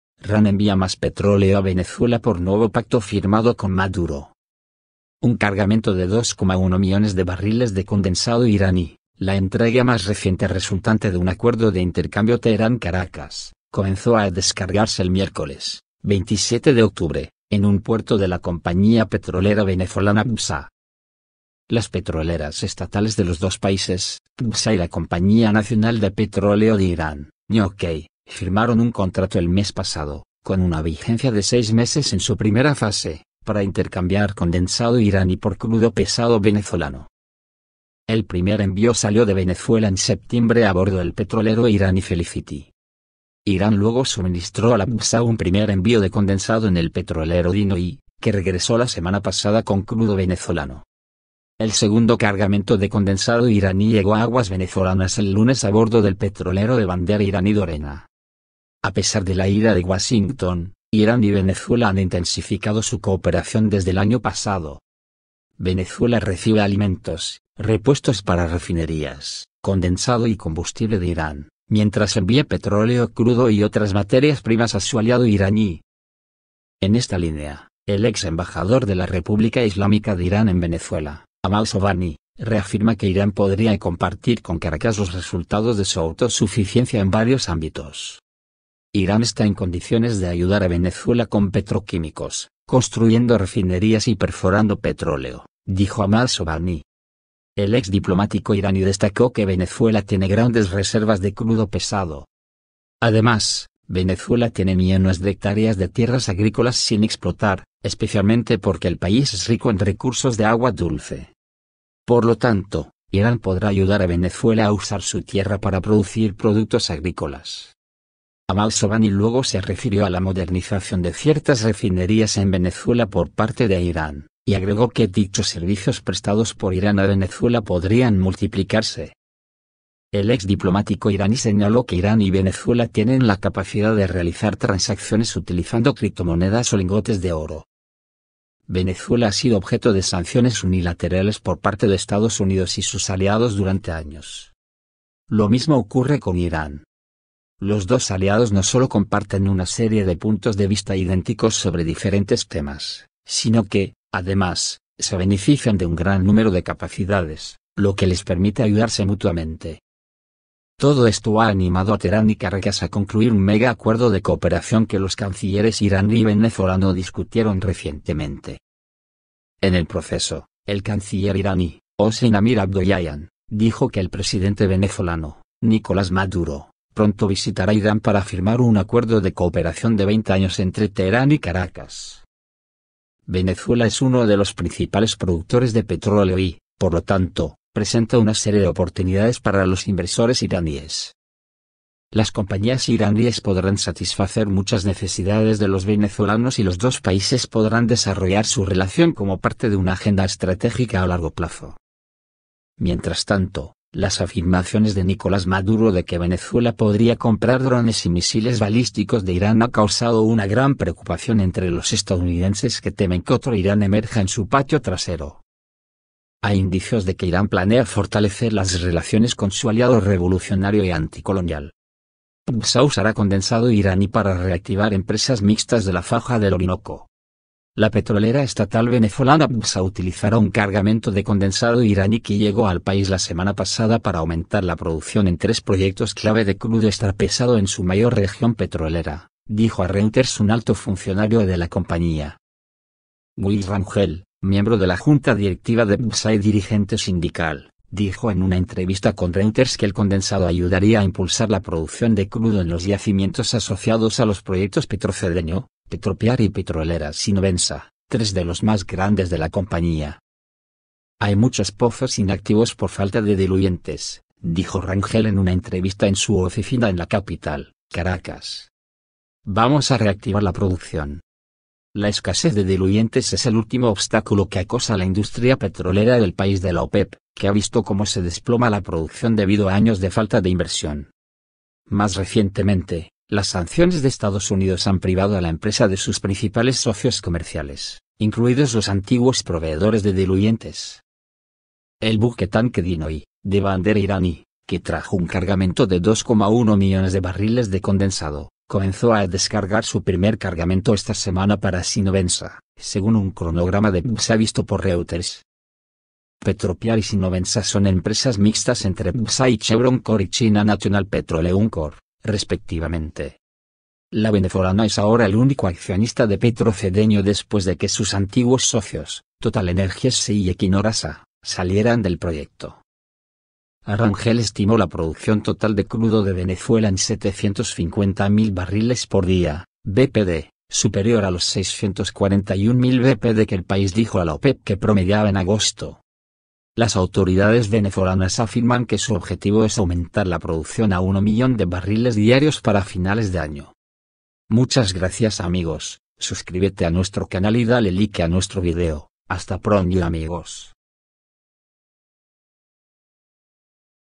IRAN AL RESCATE DE VENEZUELA , LE ENVIA TANQUERO A MADURO !
petroleros iranies  llegan  a venezuela